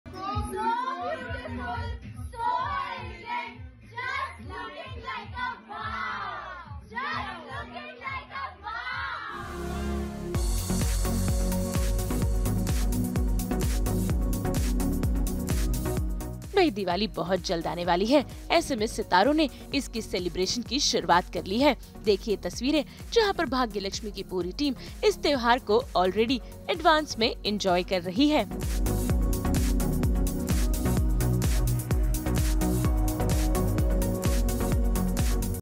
Bhagya Lakshmi team having a pre-diwali celebration at their friend's house | SBB
Bhagya Lakshmi team having a pre-diwali celebration at their friend's house